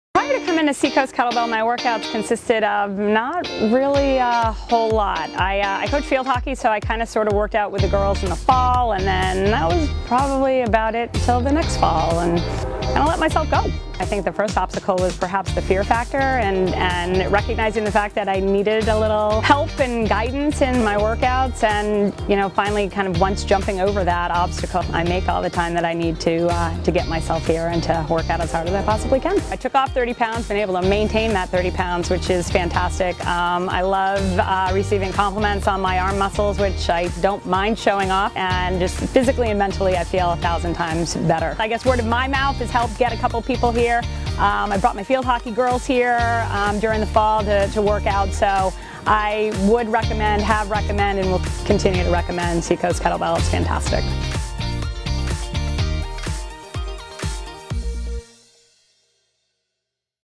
Sara Testimonial
After getting over her initial fear factor, Sara was able to get the help and guidance that she was looking for in her workouts. SKB helped Sara lose 30 pounds in the process!